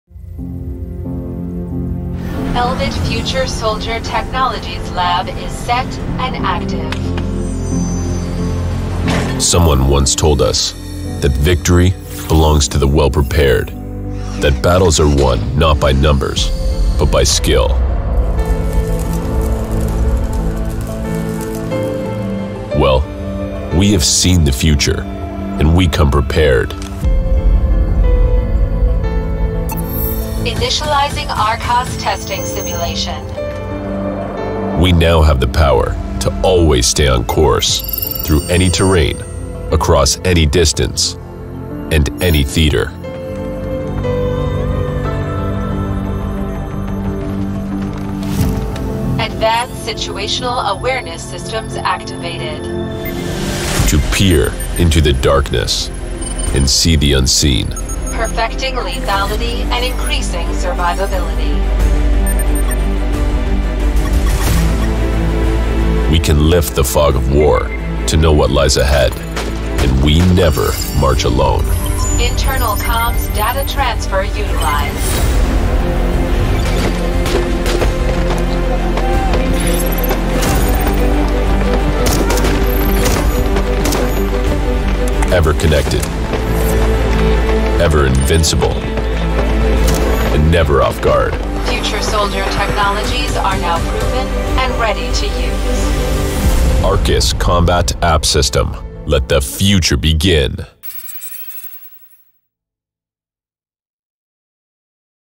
Israeli Elbit Systems Unveils ARCAS: AI-Powered, Computerized Solution for Assault Rifles
Elbit Systems unveiled Thursday the ARCAS, a built-in computerized Artificial Intelligence (AI) powered system that interfaces with the rifle’s Electro-Optical (EO) sight, with a helmet mounted eyepiece and with the rifle’s assemblies, providing soldiers with real-time intuitive actionable combat information.

ARCAS transforms assault rifles into digital, networked combat machines enabling a step change in the lethality, mission effectiveness and survivability of dismounted soldiers in both day and night.